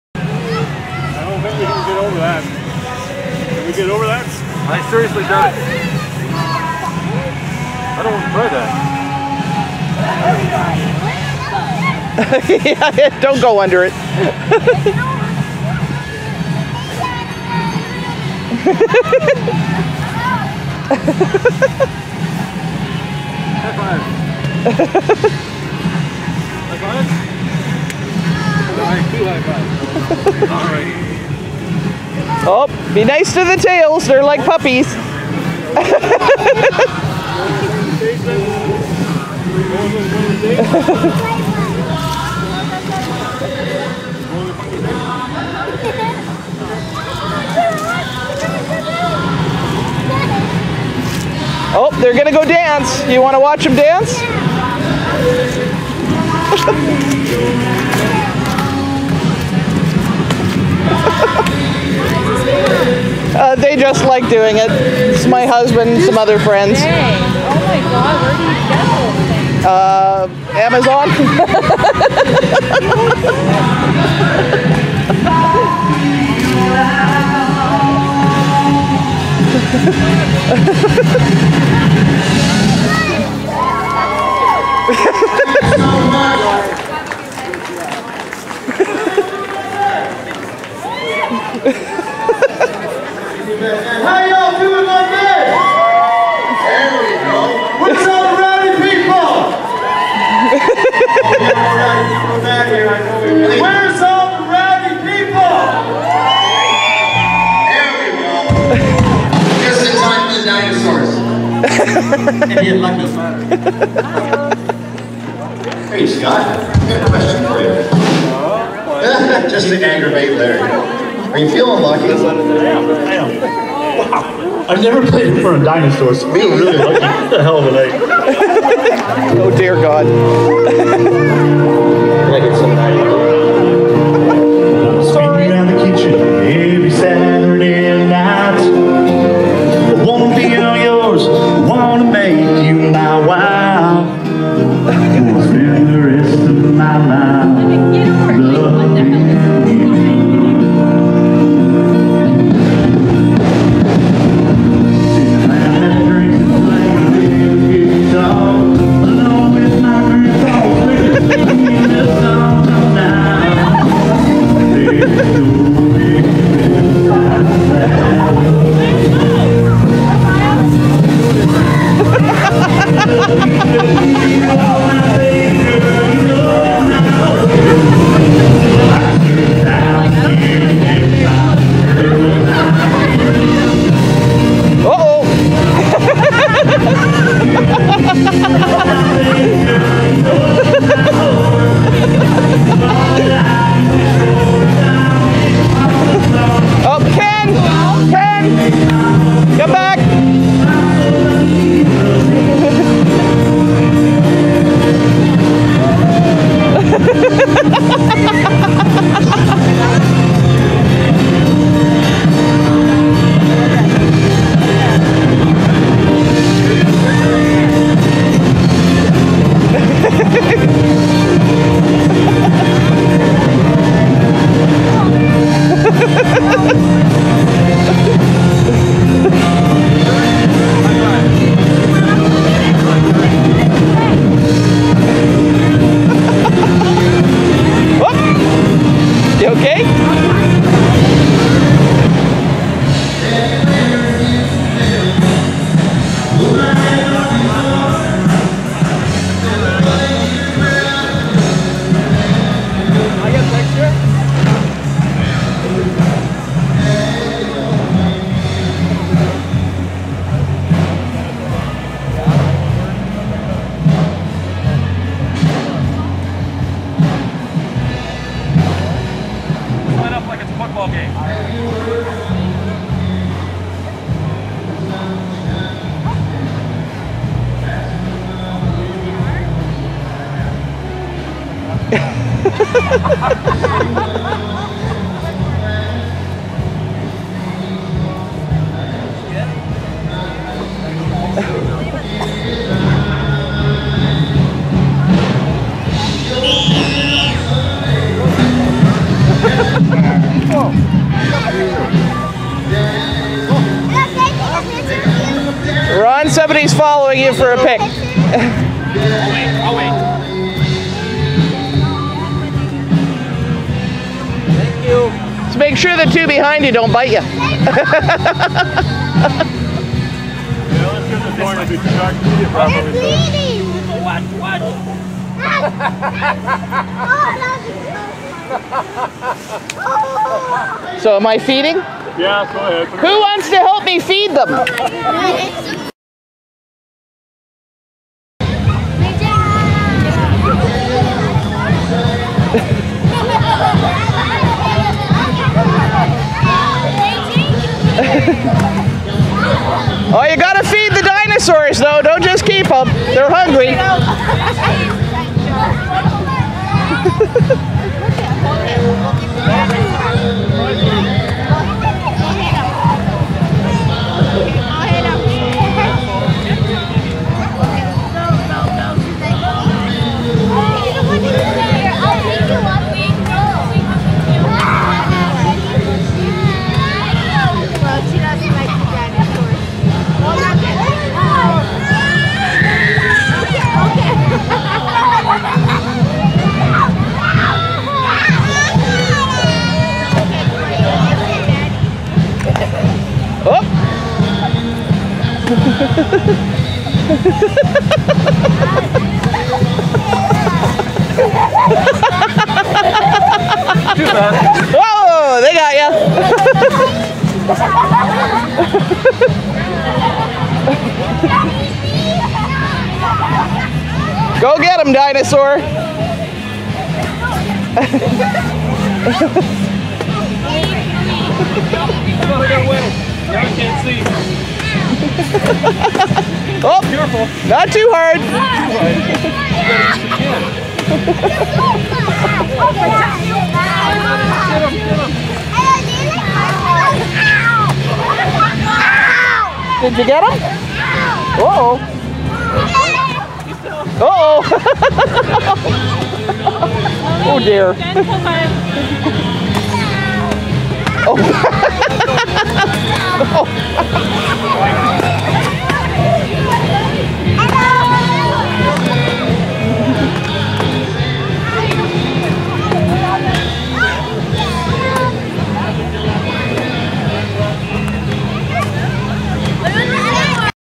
DinosauRon and friends at Feast Fest 2017
The OEV t-rex herd heads downtown to Feast Fest for a bite...
We were down a herd member for this event, and it didn't turn out quite like we planned, but it was fun nonetheless. And now, allow me to introduce... Greener the Fierce (Jennifer), and K-rex (Ken).